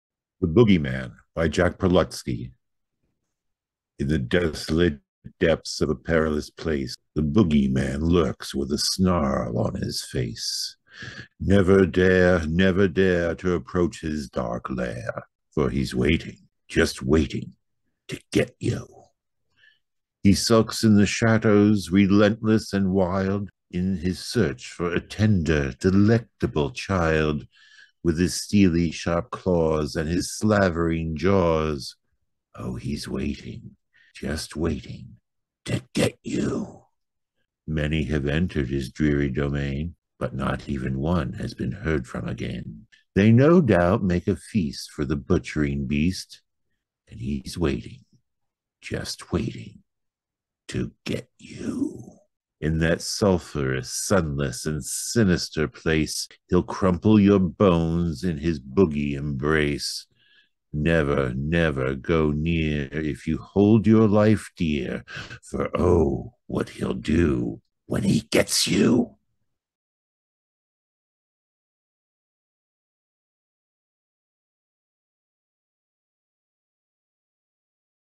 "The Bogeyman" by Jack Prelutsky
The poem "The Bogeyman" by Jack Prelutsky as read by Bob Rutan for The No Epilogue Productions' Poetry Project.

Jack Prelutsky: born 1940. American